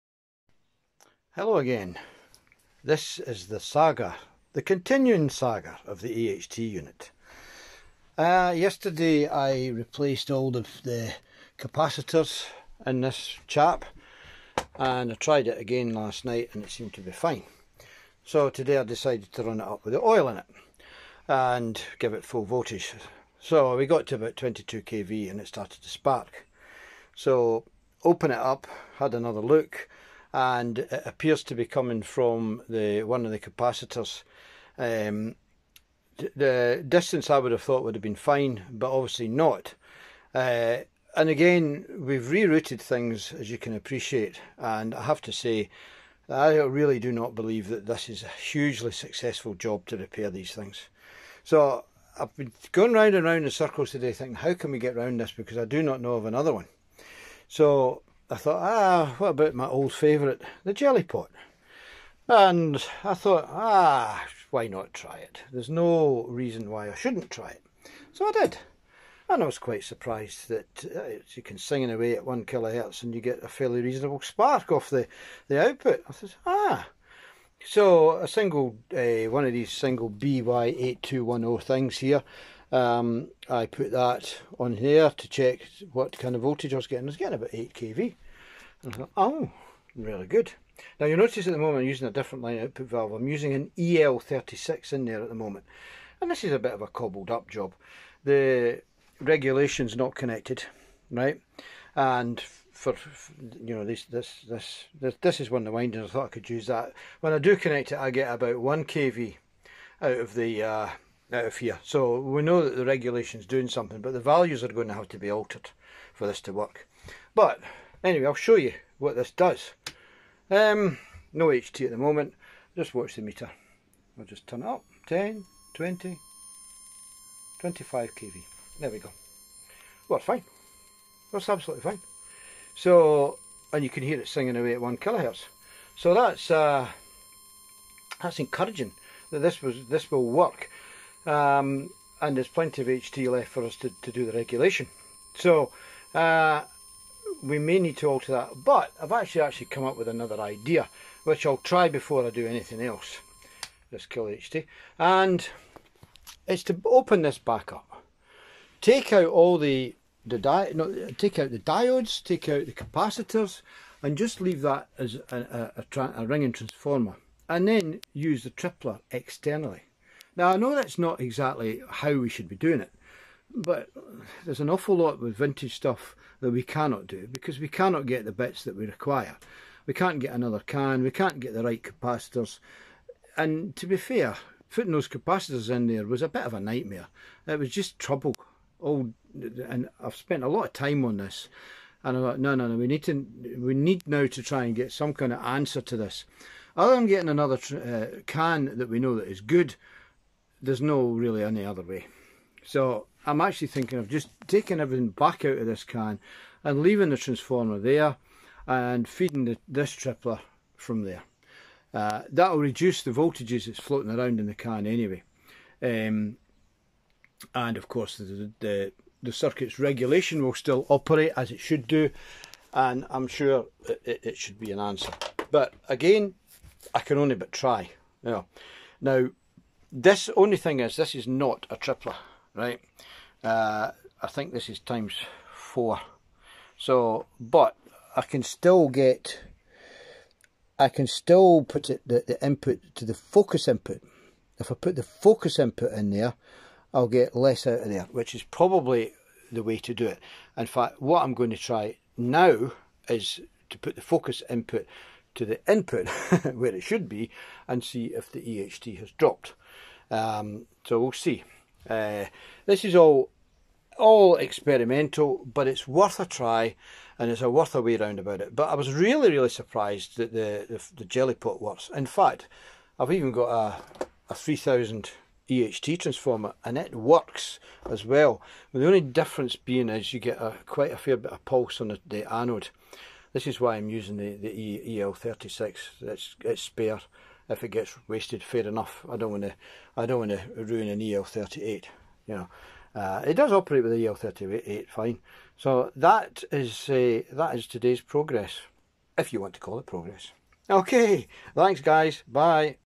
Alternative ways of generating 25kv for vintage projection TV.
After much work on the original Philips EHT transformer and internal flash over I have tried another approach.
In my comments I thought that I could feed the pulse input to the focus connection, this of course is incorrect, in the spirit of accuracy I'm just explain that it's wrong.
It is also a possibility that the multiplier may work off the original cans transformer with adjustment to the regulation circuit.